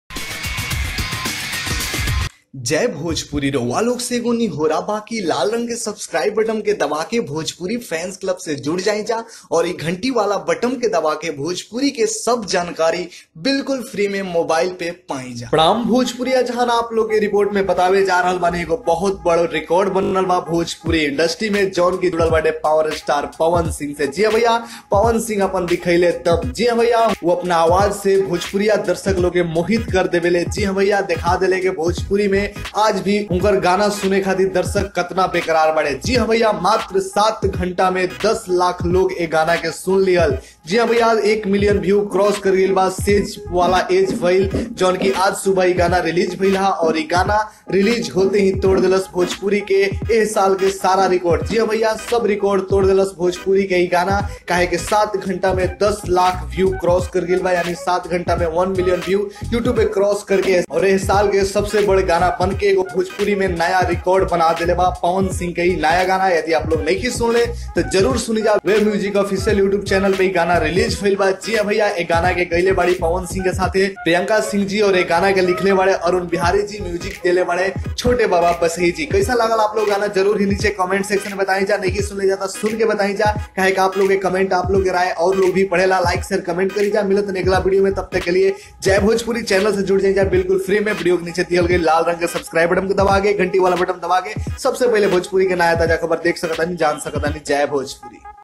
पवन सिंह ने दिखाया अपना दम, तोडा इस साल के भोजपुरी का सारा रिकॉर्ड_Pawan Singh made Record_News 2018
Credit & Thanks : - wave music